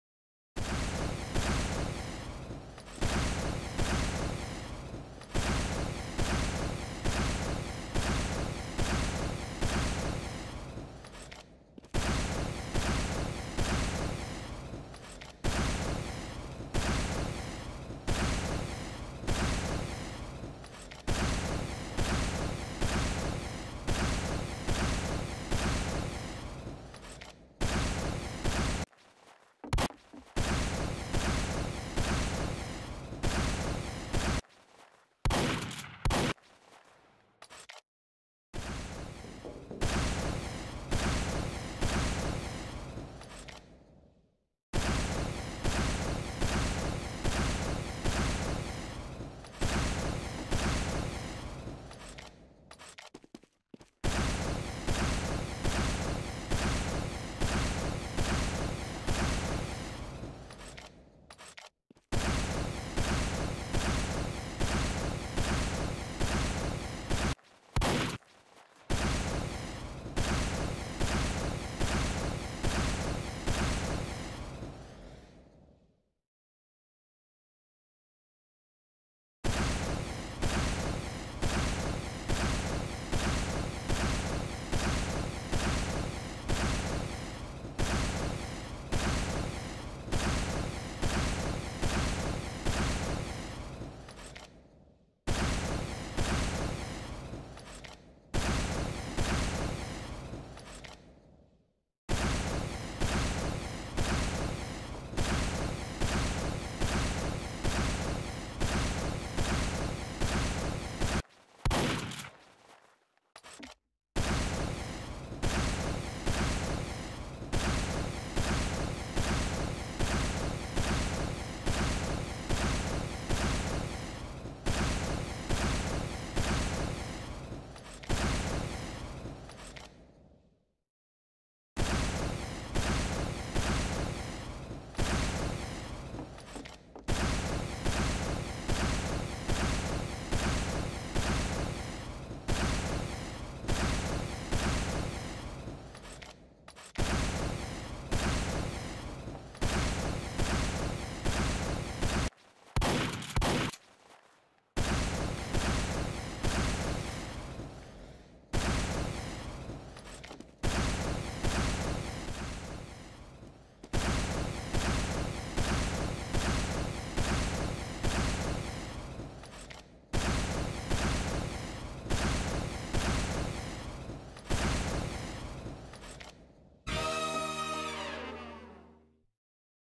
fyg on jump_bomb - 02:53.909
Player: fyg
Map: jump_bomb
Class: Soldier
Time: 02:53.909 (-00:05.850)
Date: Sat, 21 Aug 2021 19:28:29 GMT

This video was automatically generated on Sun, 22 Aug 2021 14:10:19 GMT